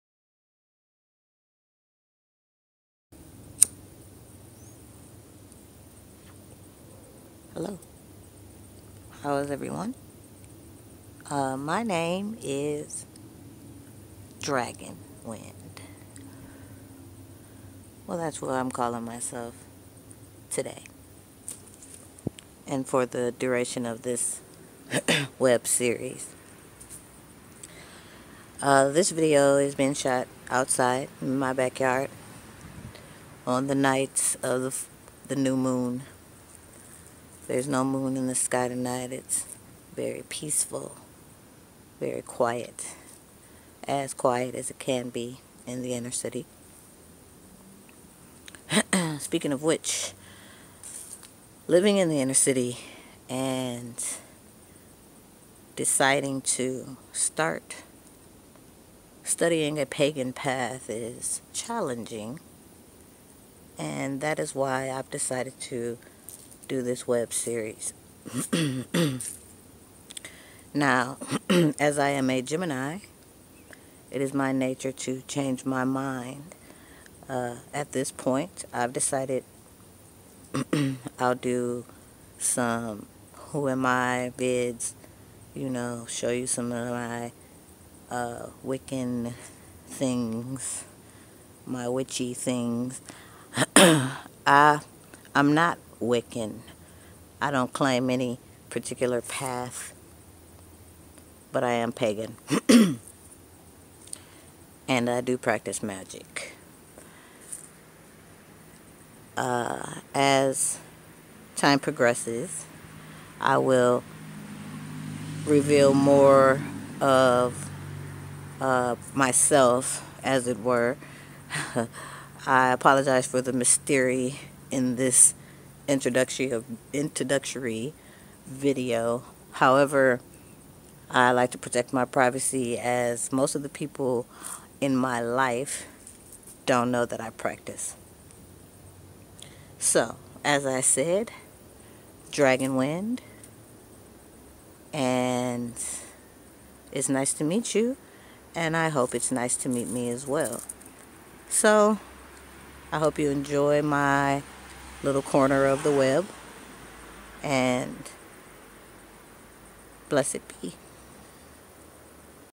Dragon Wind the Urban Pagan
This my introductory video.
Not much here other than a few thoughts on what I will be doing here.